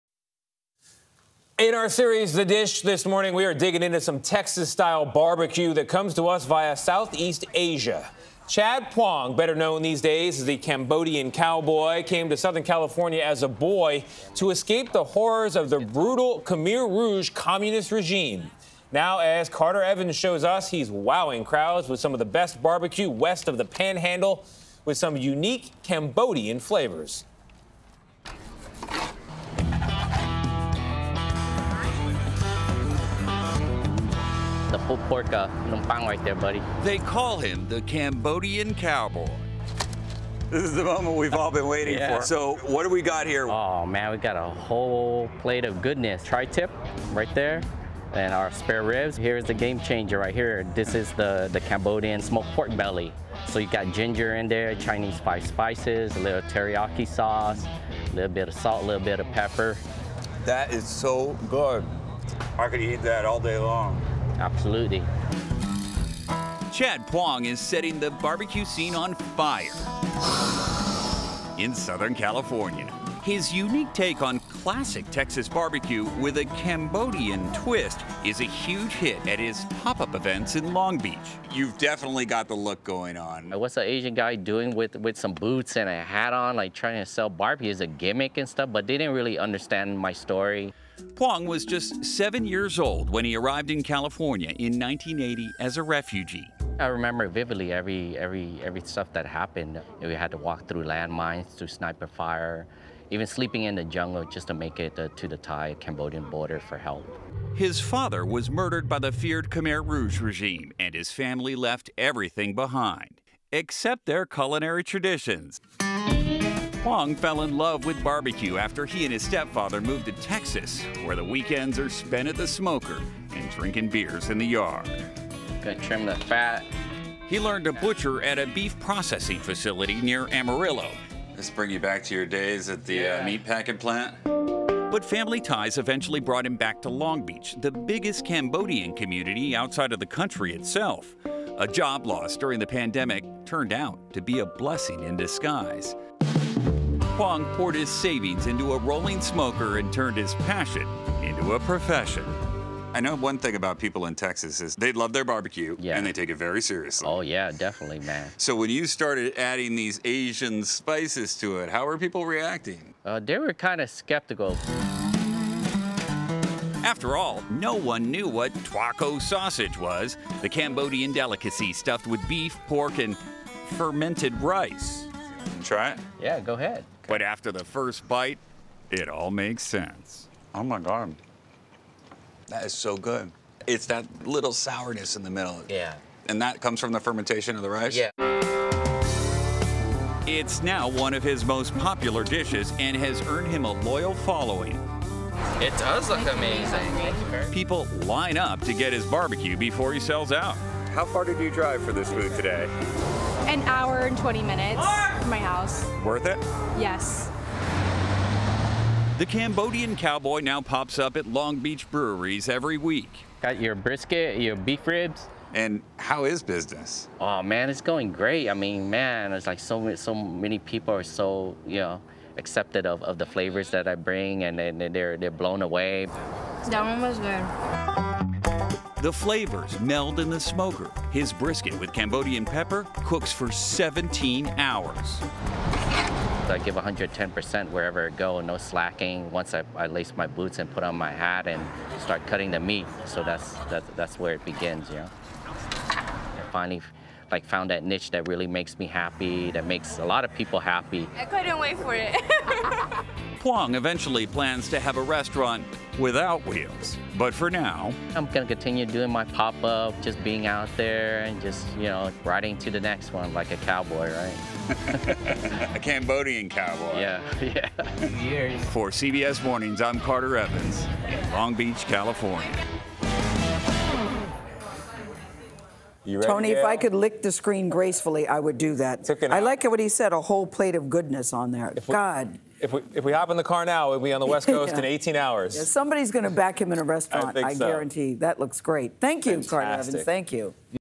Long Beach's "Cambodian Cowboy" serves up Texas BBQ with a twist
Chad Phoung came to the U.S. as a child, but it was a move to Texas as an adult that introduced him to Texas BBQ. Now the "Cambodian Cowboy" serves his Long Beach community with Texas-style BBQ with a uniquely Cambodian twist.